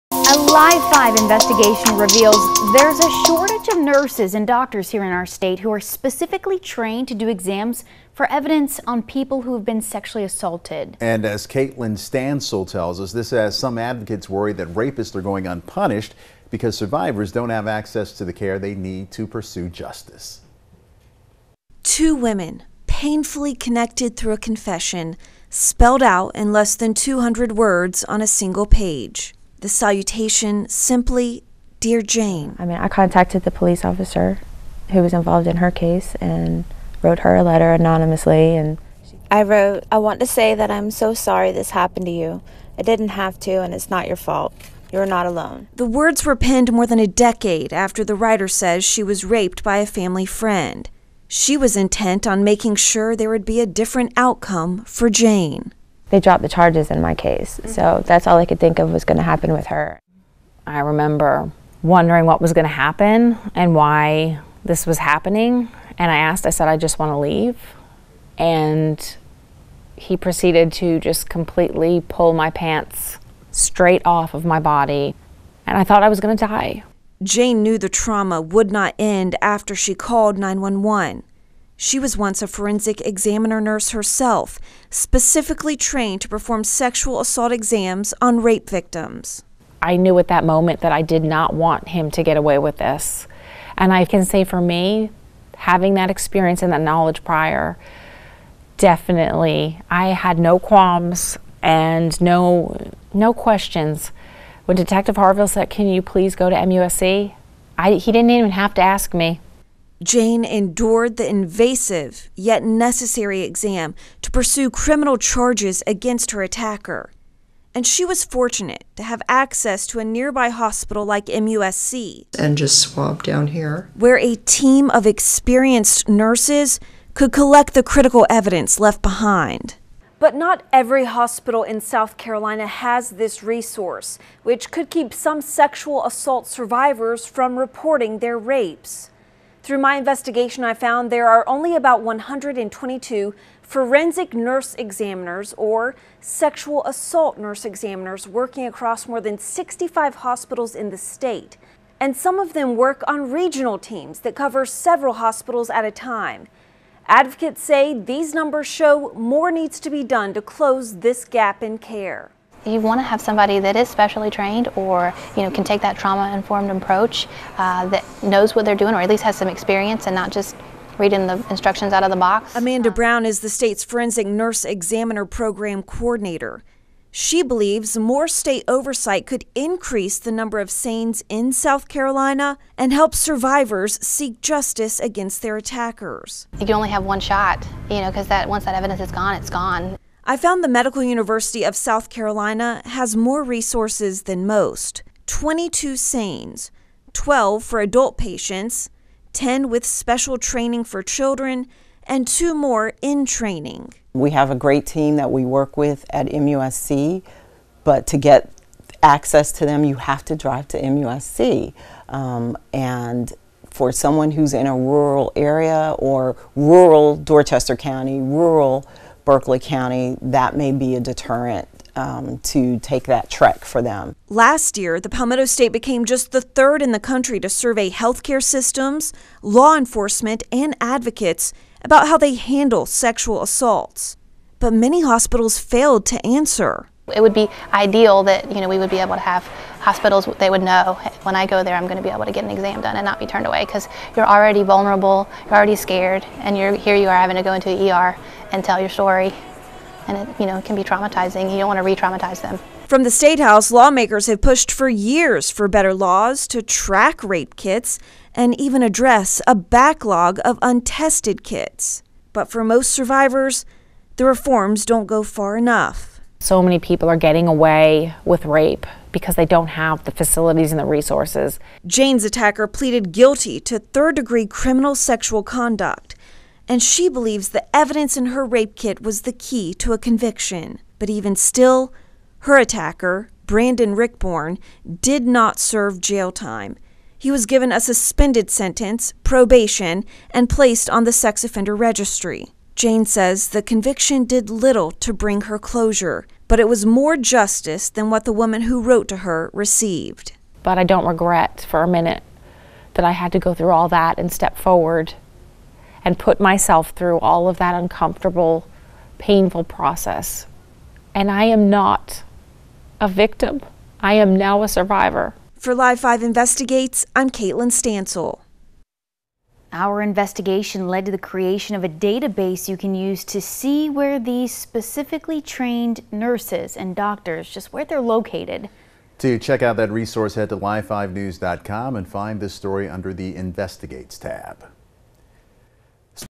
SC Shortage of Nurses Trained to Conduct Rape Kits
A shortage of nurses and doctors in South Carolina who are specially trained to conduct forensic exams on sexual assault survivors has some victim advocates worried that rapists are going unpunished because survivors don’t have access to the resources they need to pursue justice.

A mapped database of data collected from 65 hospitals across the state details which hospitals employee forensic nurse examiners or sexual assault nurse examiners who are certified and trained to collect the critical evidence left behind after a sexual assault.